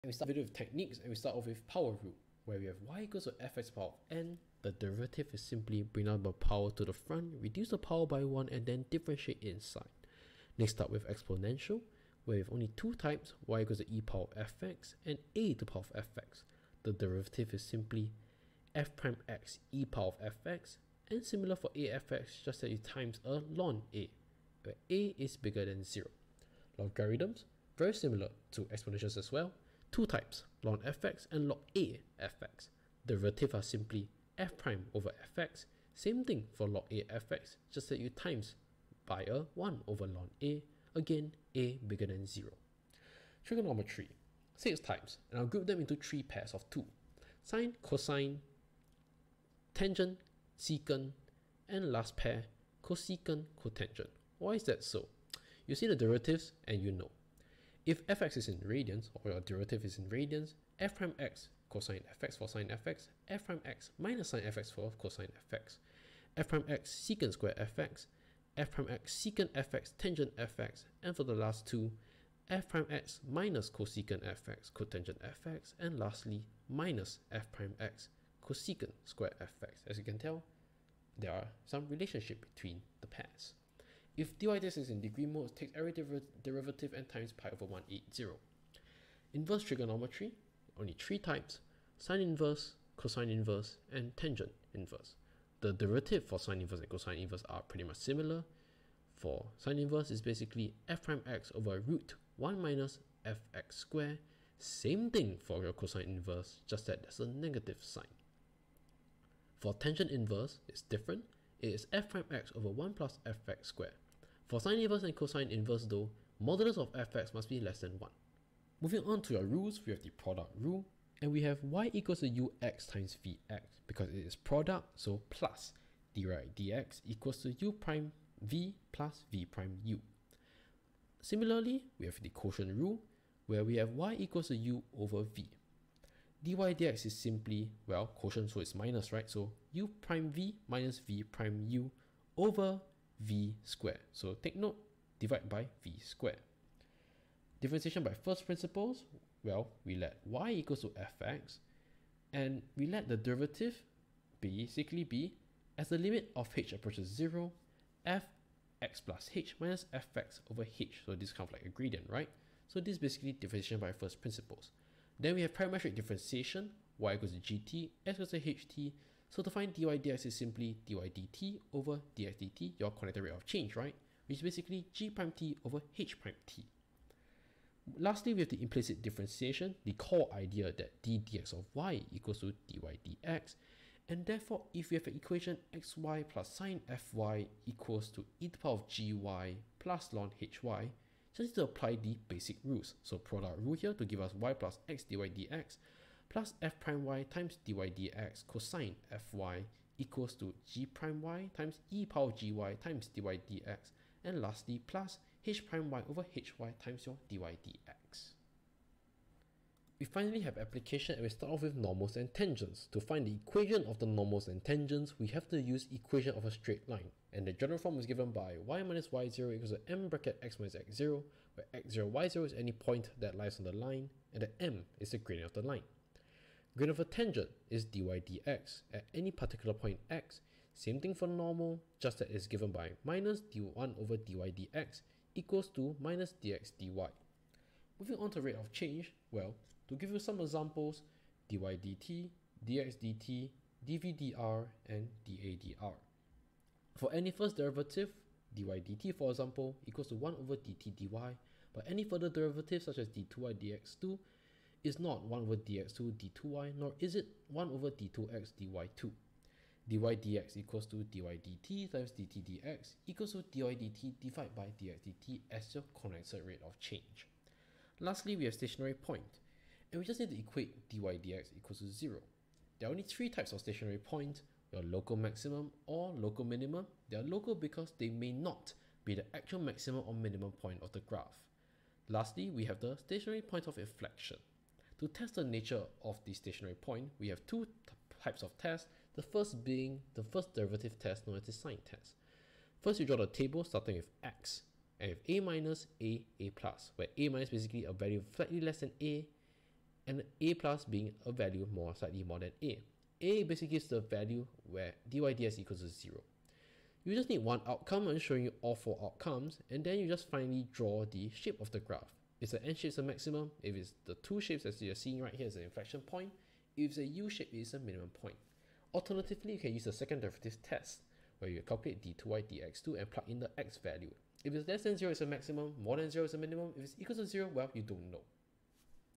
Crash Course/Summary: H2 Math Differentiation in under 5.5 minutes
Hai~ :3
Enjoy this crash course/summary video for H2 Math Differentiation